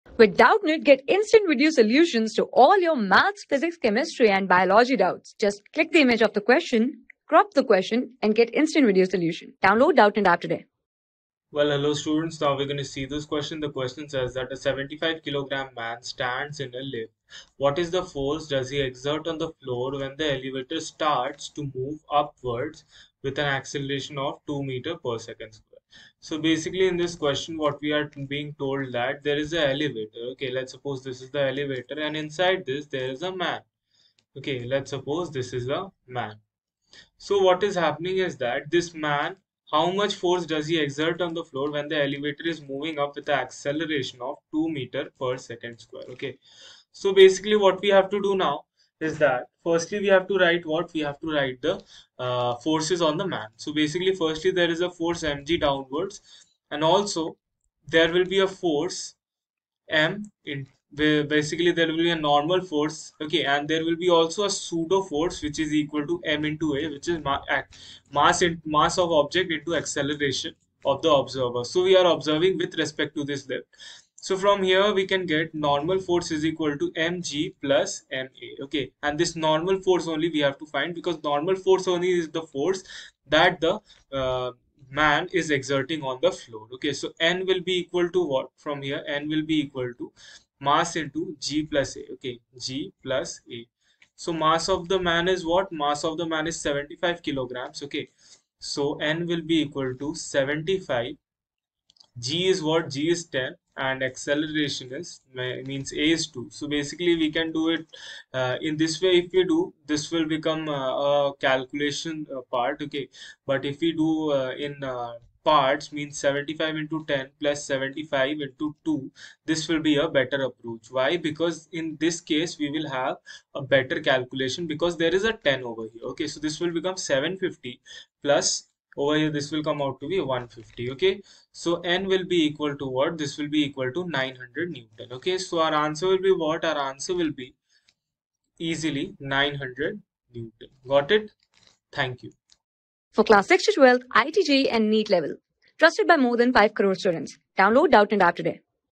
A 75kg man stands in a lift. What force does the floor exert on him when the elevator starts mo...
A 75kg man stands in a lift. What force does the floor exert on him when the elevator starts moving upward with an acceleration of 2 ms^(-2) Given: g = 10 ms^(-2).
Class: 12
Subject: PHYSICS
Chapter: LAWS OF MOTION
Board:IIT JEE

👉 Have Any Query? Ask Us.